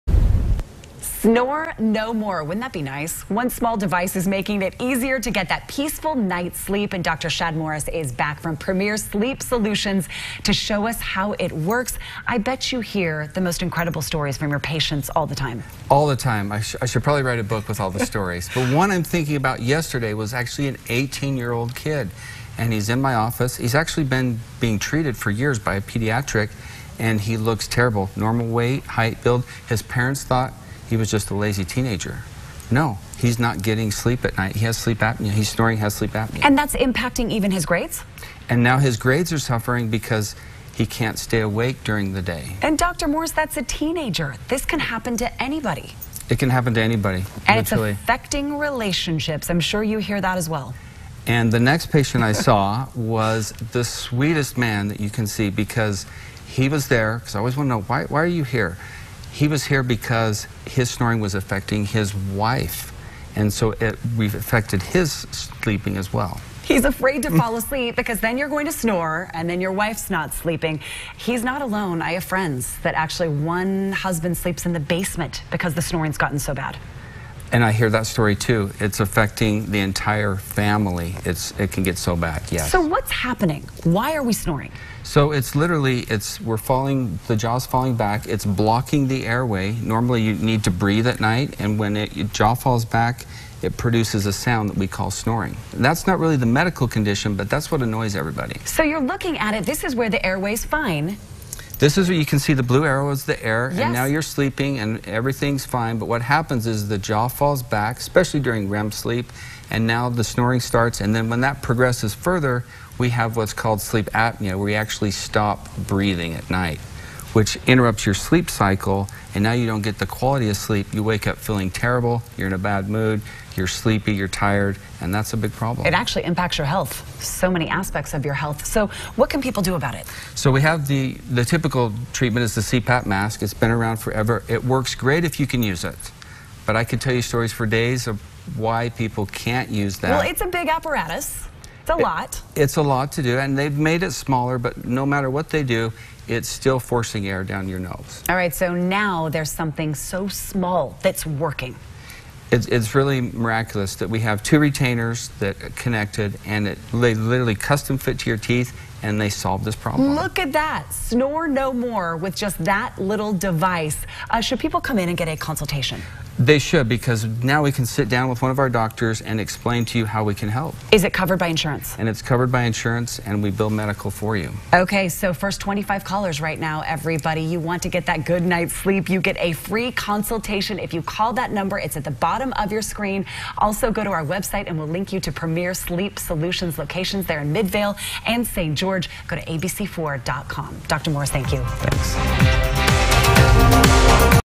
Premier Sleep Solutions Midday Jan 2019
Dr. Shad Morris at Premier Sleep Solutions is pioneering another option for those who have sleep apnea but aren’t able to adjust to a CPAP machine.